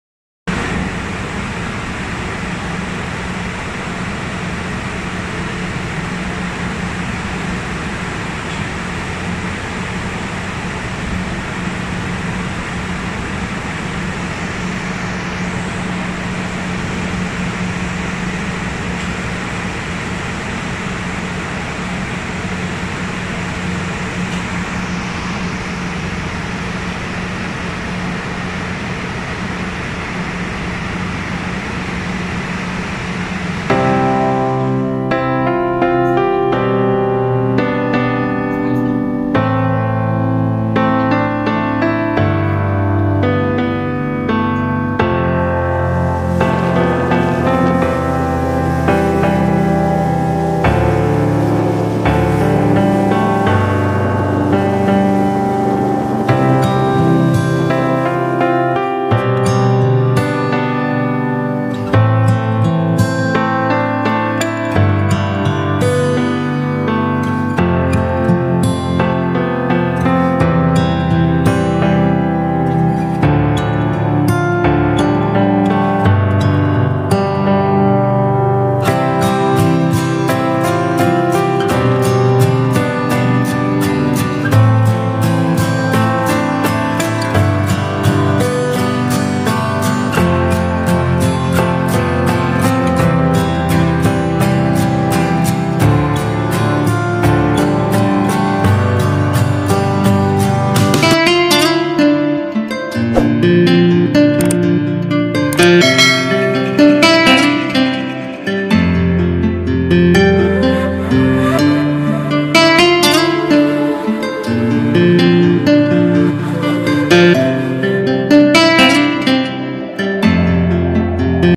Ship life | rough sea | life at sea | SAUDI ARAMCO OIL FIELD | DP 2 vessel | Sailor Life
Vandematram guys...
Here are some glimps of rough weather I have encountered.
ther are so many ups and downs we encounter during voyages still less known professionals..

I hope the day will come soon when everyone will recognize our contributions too..
I am trying  to show you the diffrence phases during sailing cuz there are so may myths in the air about merchant navy profession which needs to be clarified...

For Any Quarries
Do Comment  or can follow me On.